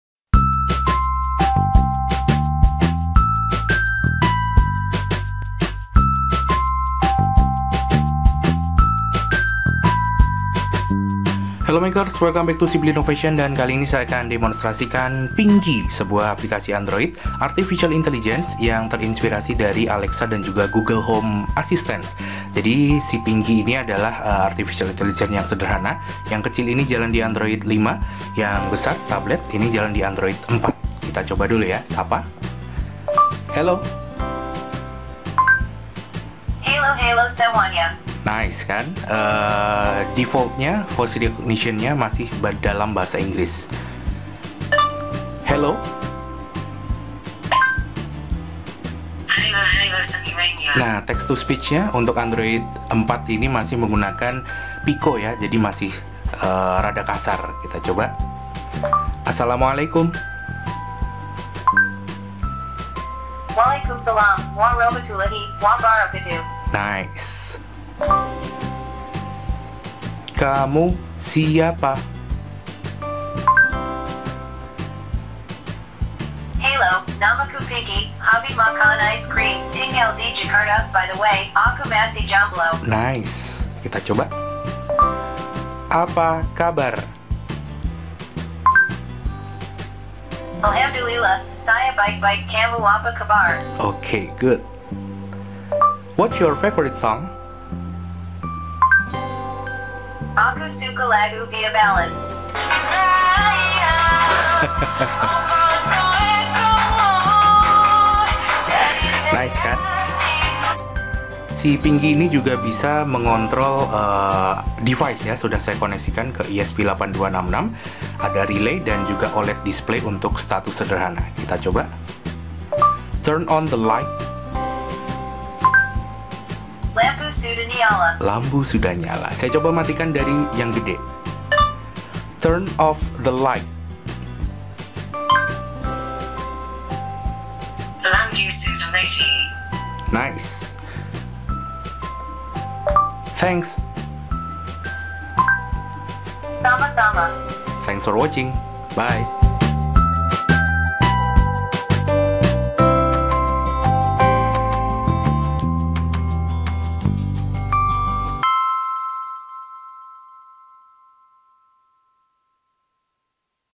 Pinky: Simple AI & IoT Project with ESP8266
Inspired by Alexa & Google Home, I built a simple experiment on AI (artificial intelligence) & IoT (internet of things) named Pinky. It works on Android mobile app using Google voice recognition & text to speech engine, with AI database on PHP. For hardware, I used a NodeMCU ESP8266 board, a 0.96" OLED display & a relay.